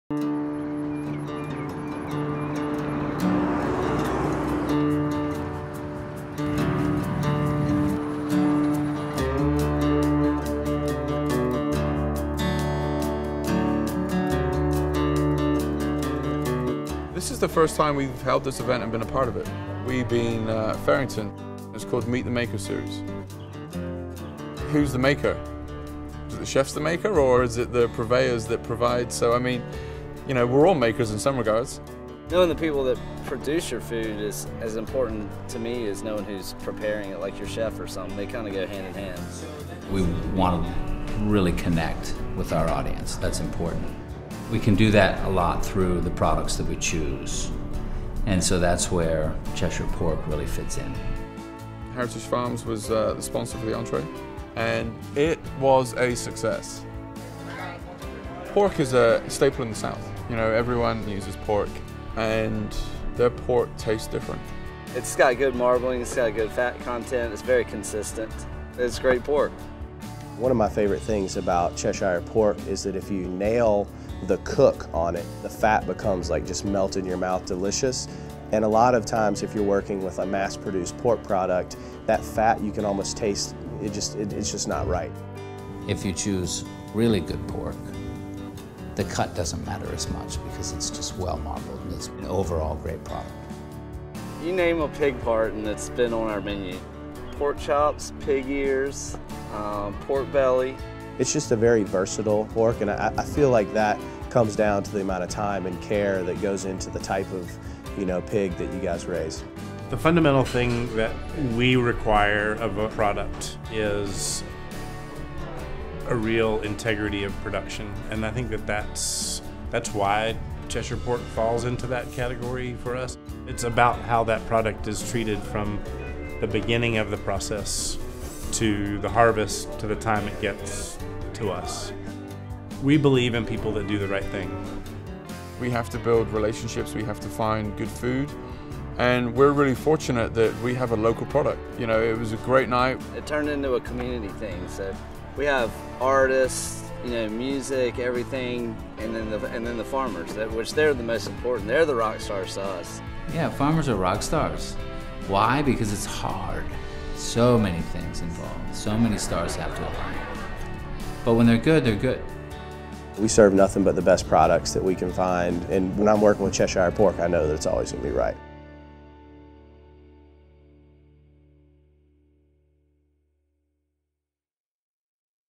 Cheshire Pork: Meet the Maker Dinner at Fearrington House Restaurant
The Fearrington House Restaurant and Chef Colin Bedford feature Heritage Farms Cheshire Pork in a special "Meet the Maker" farm-to-table dinner along with other award-winning North Carolina chefs.